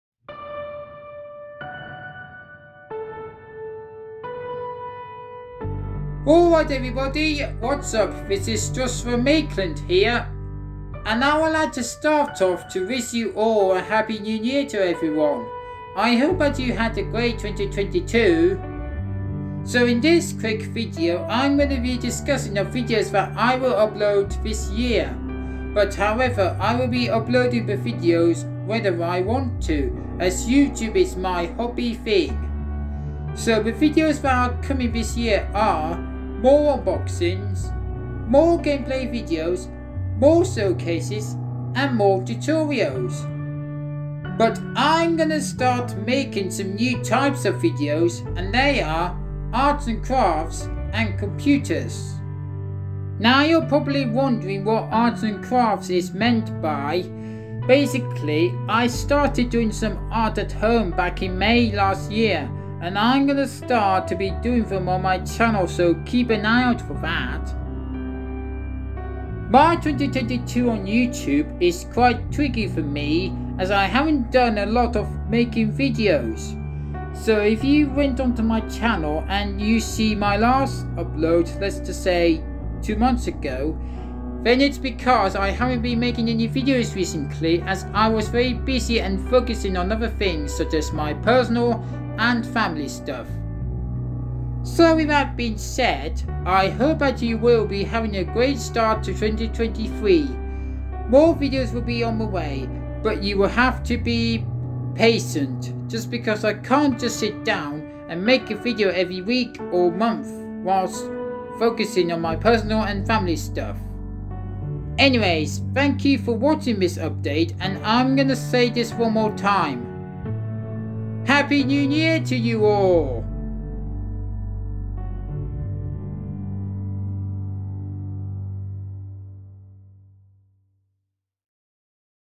New Year, New Videos (2023)
This video will explain about what is coming to my channel in 2023.